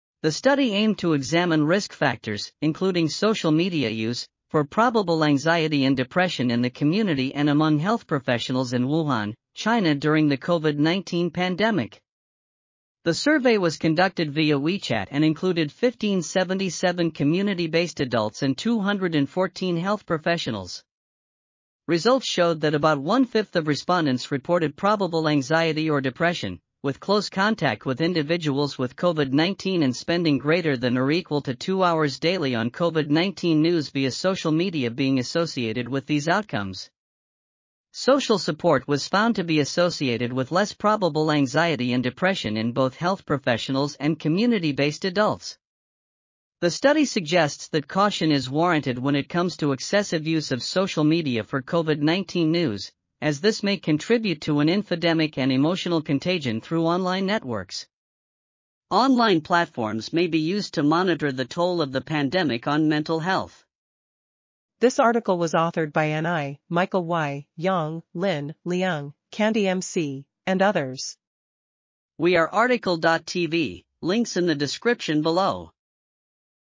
Mental Health, Risk Factors, and Social Media Use During the COVID-19 Epidemic and Co... | RTCL.TV
### Keywords ###

### Article Attribution ###
Title: Mental Health, Risk Factors, and Social Media Use During the COVID-19 Epidemic and Cordon Sanitaire Among the Community and Health Professionals in Wuhan, China: Cross-Sectional Survey
Authors: Ni, Michael Y, Yang, Lin, Leung, Candi M C, Li, Na, Yao, Xiaoxin I, Wang, Yishan, Leung, Gabriel M, Cowling, Benjamin J ,and Liao, Qiuyan

### Video Timestamps ###
0:00:00 - Summary
0:01:04 - Title
0:01:11 - Tail
0:01:16 - End